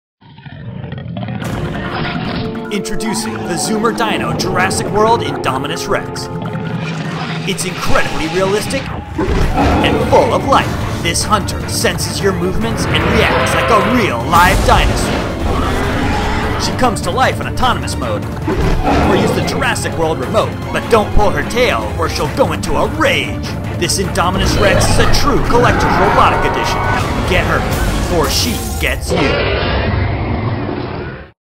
Spin Master | Zoomer Dino Jurassic World - Indominus Rex
Bring home the adventure of Jurassic World™ with the incredible Collectible Robotic Edition Zoomer Dino, Indominus rex™! Unleash her stealthy, aggressive and unpredictable nature. Interact with her as she prowls your home, using True Balance Technology™ to hunt, patrol and explore her surroundings while balancing on two wheels. With advanced IR nose sensors, Indominus rex™ can sense your hand and will respond to your movements. Featuring real movie sounds, color changing eyes that communicate mood, a customized Jurassic World™ remote control and a numbered certificate of authenticity, Indominus rex™ is the most hauntingly realistic Zoomer Dino around!

Follow Zoomer!